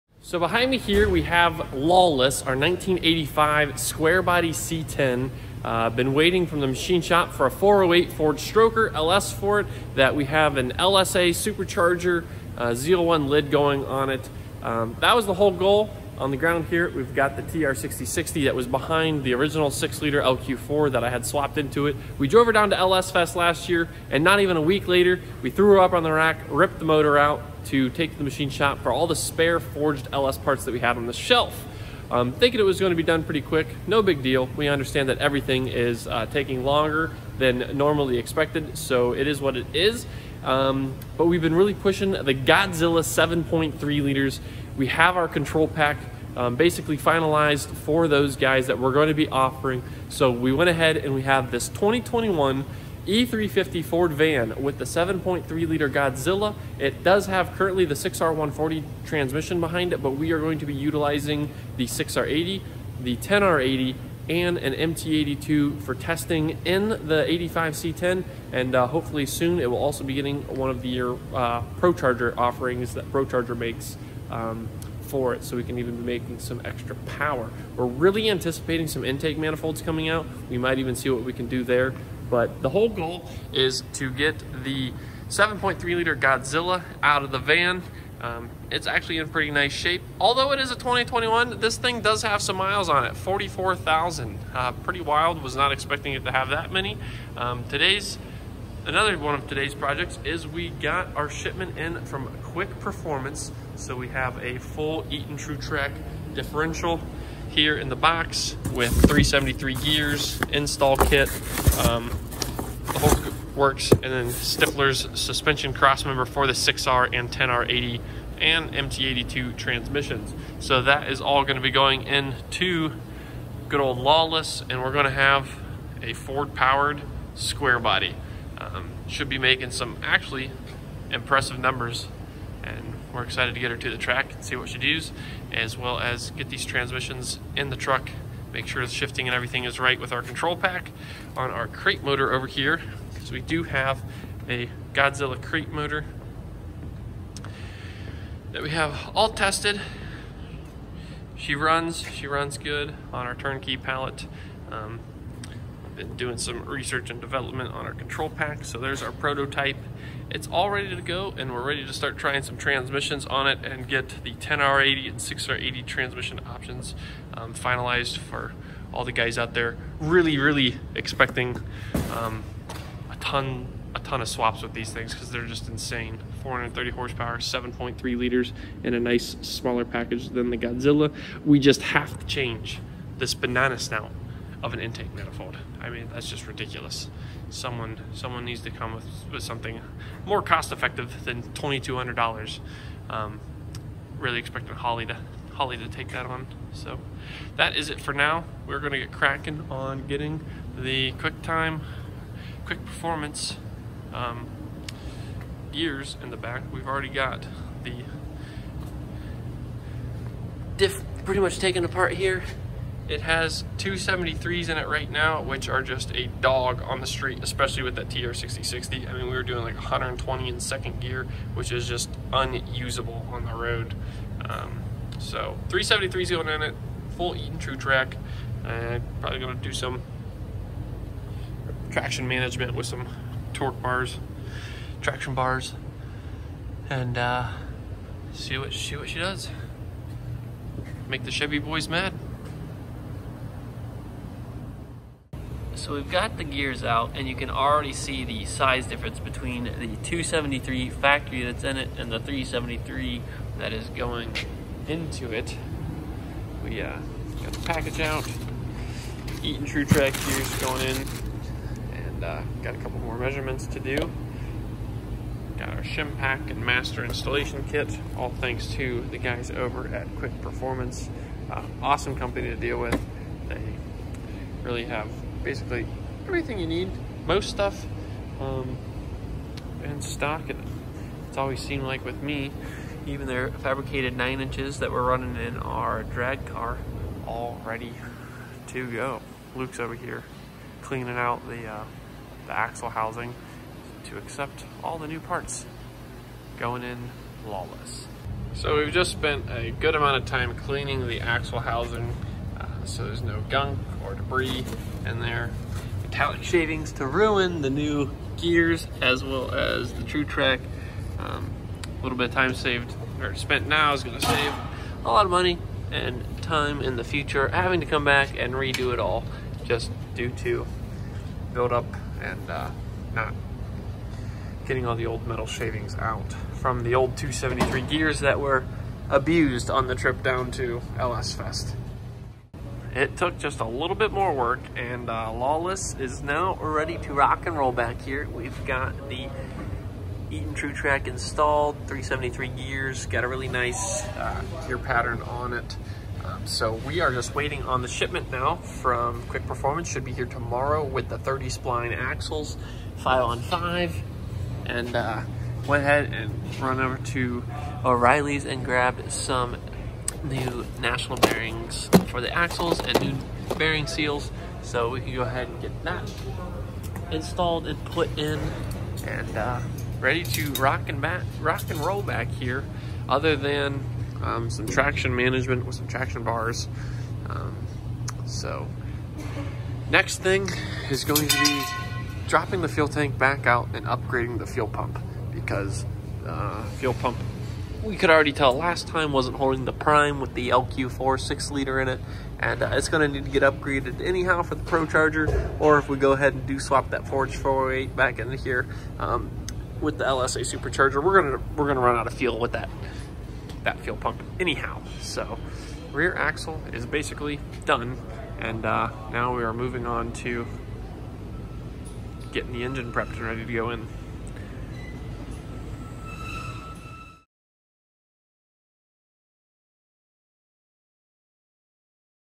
GODZILLA 7.3L Swapped Chevy Squarebody C10
This week we randomly decided to swap my 1985 C10 with a Godzilla 7.3L while we still wait on the forged 408ci stroker motor from the machine shop. This swap project is intended for us to utilize the test mule for control-pack research and developement to cover the popular 6R80 and 10R80 transmissions behind the 7.3L GODZILLA!!!